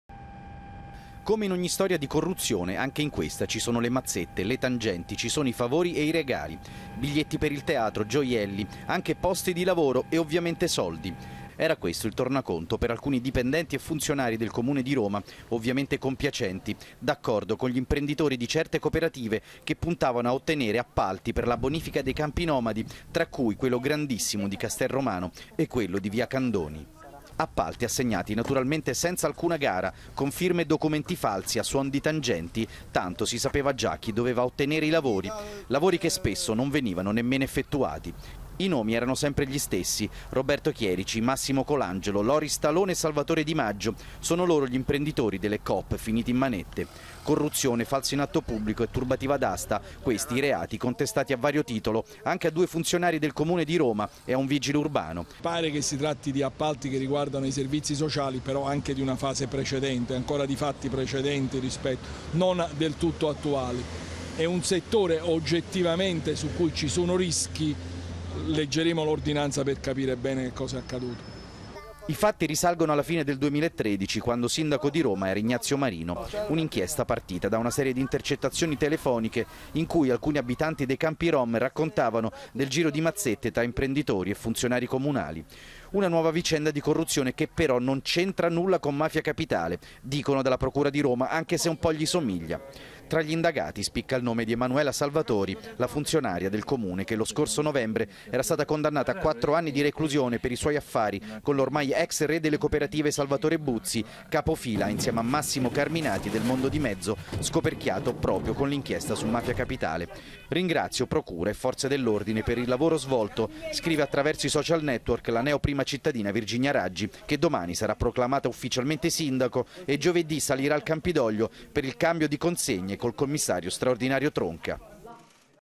Roma: dipendenti del Comune e imprenditori in manette per un giro di tangenti
Con la cronaca rientriamo in Ttalia. di nuovo roma corrotta. di nuovo dipendenti del comune e imprenditori in manette per un giro di tangenti. e di nuovo appalti per i lavori nel sociale, in questo caso la manutenzione dei campi nomadi. Un'inchiesta che non ha alcun legame con mafia capitale, e forse questa è una aggravante. Servizio di Federico Plotti e l'intervista a Raffaele Cantone